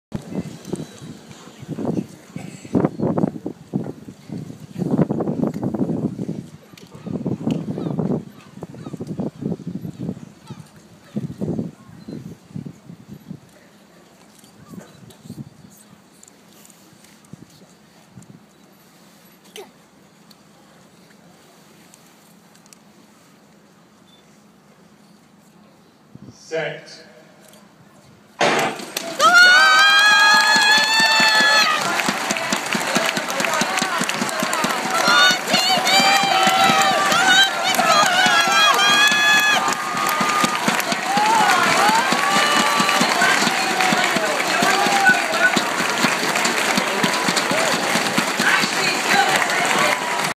Aviva Birmingham Grand Prix 200m men's final - Tyson Gay, Ryan Bailey, Christian Malcom...
Diamond League Men's 200m event competed by some fantastic athletes! Top three racers:

Nickel Ashmeade - 20.12
Tyson Gay - 20.21
Wallace Spearmon - 20.23